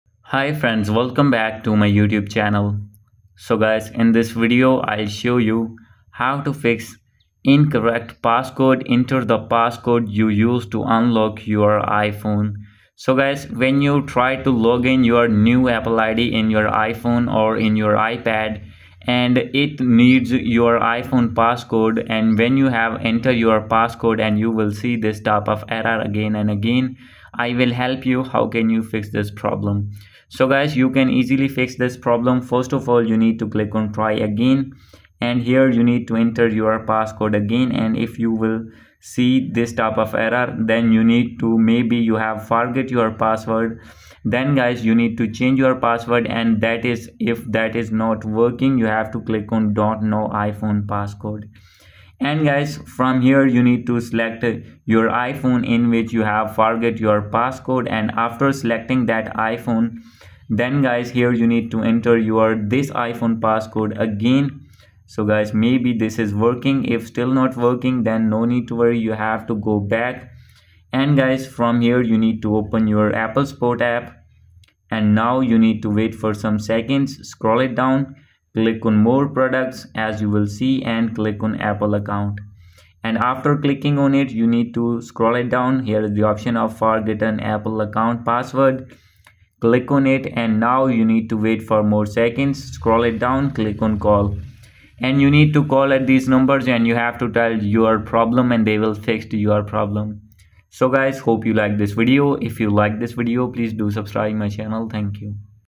Incorrect Passcode: Enter the passcode you use to unlock your iPhone | How to Fix
Welcome to our step-by-step guide on how to fix the 'Incorrect Passcode: Enter the Passcode You Use to Unlock Your iPhone' error. 📱✨ In this video, we will walk you through the process to resolve this common issue, ensuring you can access your device without any hassle. Whether you’ve forgotten your passcode or you're facing this error for other reasons, we’ve got you covered. Follow along as we simplify each step to help you regain access to your iPhone.